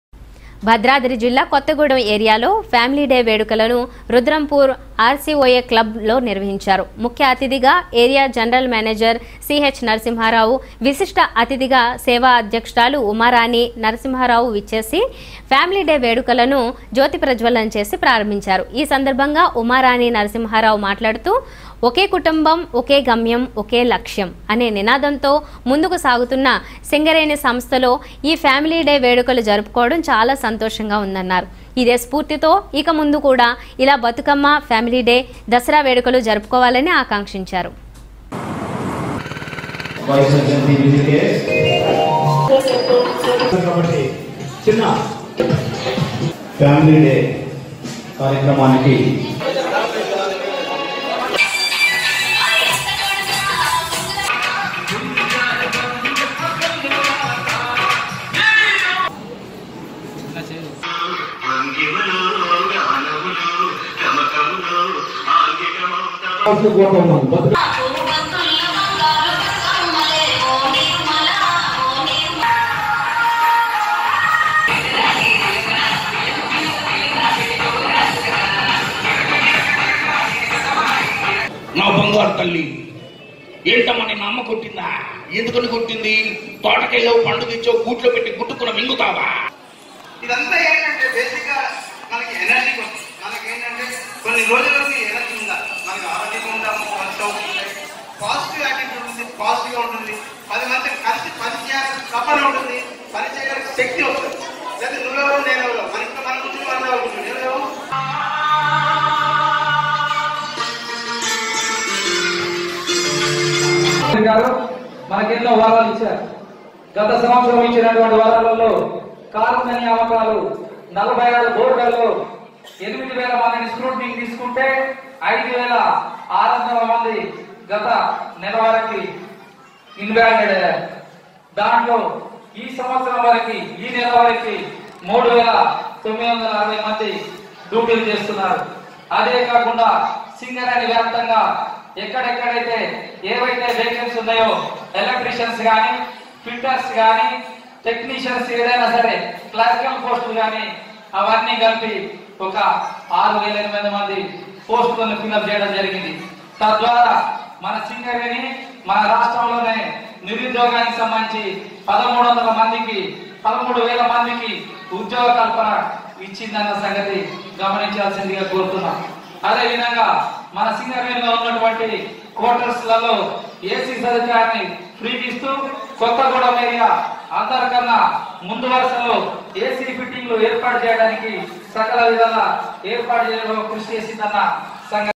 భద్రాద్రి జిల్లా కొత్తగూడెం ఏరియాలో ఫ్యామిలీ డే వేడుకలను రుద్రంపూర్ ఆర్‌సి‌ఓ‌ఏ క్లబ్లో నిర్వ‌హించారు
భద్రాద్రి  జిల్లా కొత్తగూడెం ఏరియాలో ఫ్యామిలీ డే వేడుకలను  రుద్రంపూర్ ఆర్‌సి‌ఓ‌ఏ క్లబ్లో నిర్వ‌హించారు.  ముఖ్య అతిధిగా ఏరియా జనరల్ మేనేజర్ సి‌హెచ్.నరసింహ రావు,  విశిష్ట అతిధిగా సేవ అధ్యక్షురాలు   ఉమారాణి నరసింహా రావు విచ్చేసి ఫ్యామిలి-డే వేడుకలను జ్యోతి ప్రజ్వలన చేసి ప్రారంభించారు.  ఈ‌ సందర్భంగా   ఉమారాణి నరసింహా రావు మాట్లాడుతూ... ఒకే కుటుంబం, ఒకే గమ్యం, ఒకే లక్ష్యం అనే నినాదంతో ముందుకు సాగుతున్న సింగరేణి సంస్ధలో ఈ ఫ్యామిలి డే వేడుకలు జరుపుకోవడం చాలా సంతోషంగా ఉందన్నారు. ఇదే స్ఫూర్తితో ఇక ముందు కూడా ఇలా బతుకమ్మ, ఫ్యామిలి డే ,  దసరా వేడుకలు జరుపుకోవాలని  ఆకాంక్షించారు.